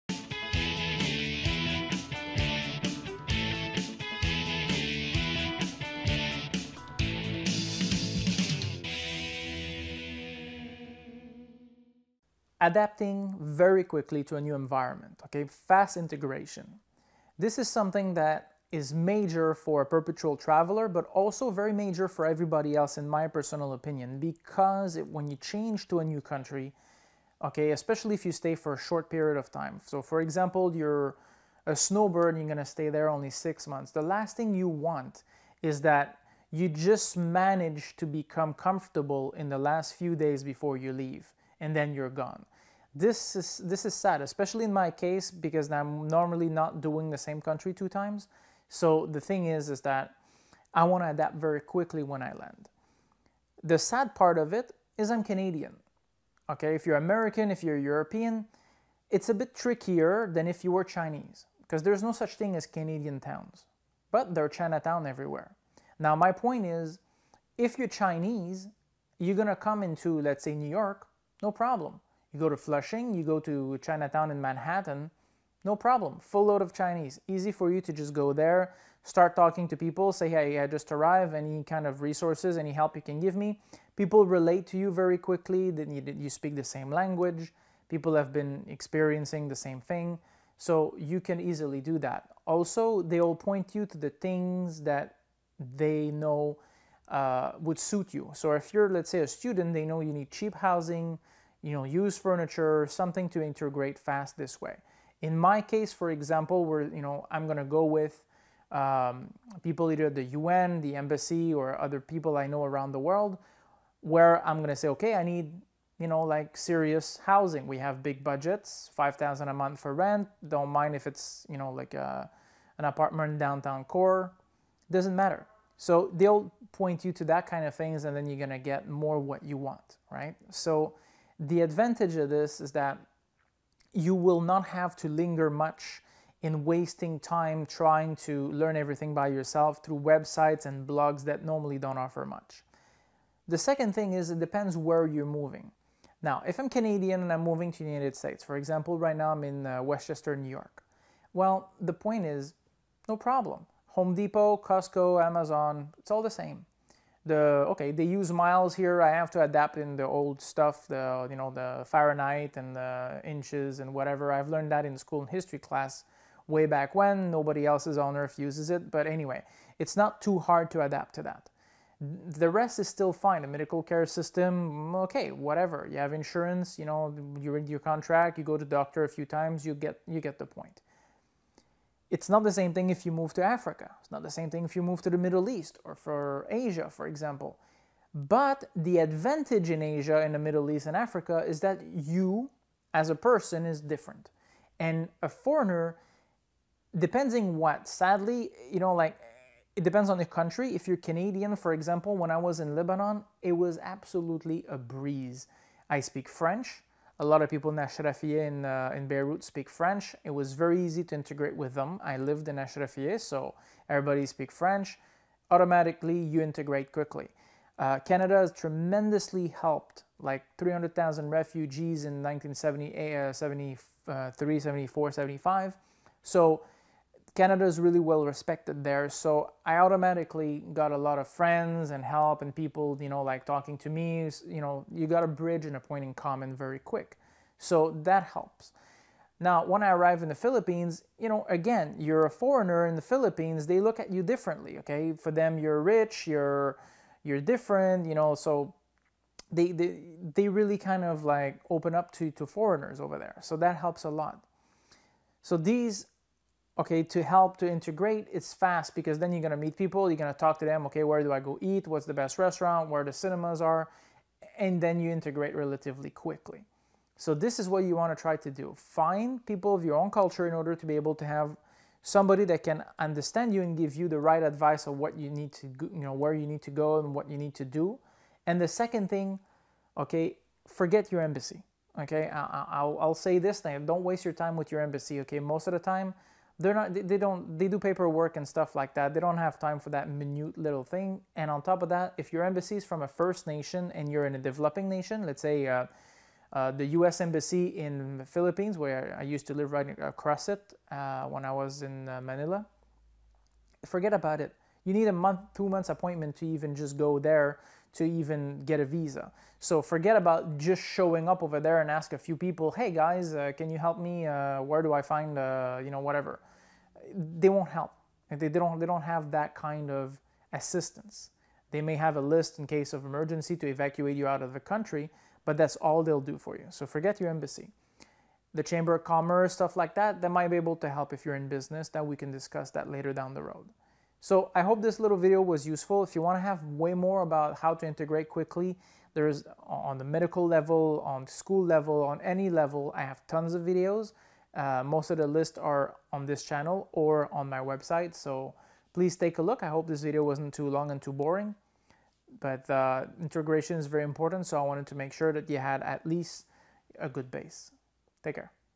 Fast Integration - Less Culture Shock
Adapting very quickly to a new environment is extremely important for both perpetual travellers and tourists alike, especially if you only get to visit for a short period of time.

The last thing you would want to take out of your experience in a completely different country is to manage to get comfortable only in the last few days of your trip. By the time you are finally accustomed to the cultures, traditions and everything else it has to offer, you are gone before you even know it.

In this video, Mario Stinger talks about some of his experiences as well as a few tips   to avoid this from happening and to help you integrate faster when staying in a foreign country.

Finding people of your own culture, or those who you can easily speak to or relate with, is the first thing he recommends for a relatively quicker and easier integration. This way, you will be able to directly ask them for help in navigating and learning your way around the country, without wasting your time researching about them in blogs and other websites that normally do not offer much guidance and direction.

He also shares that fast integration can depend on the country you are going to. Some places have common necessities, laws and customs that are easily accessible and relatable, while there are also a few areas where people native to the country view different foreigners in a way that can actually help your moving in become a breeze. In Mario’s case, some of the places he has visited either have a good history and relationship with his home country, or generally think highly of foreigners, helping him connect with both the people and the country much faster.

Another tip, he says, is not to depend on your country’s embassy for help on simple things, especially if you are staying in a developing nation. According to him, you will only waste your time asking them for help about basic matters, such as housing and what not, as they do not offer much assistance and will also take you a month or two to even set up an appointment with them.

Click the link below for more information about how to integrate faster in medical, educational and other aspects when living or staying abroad.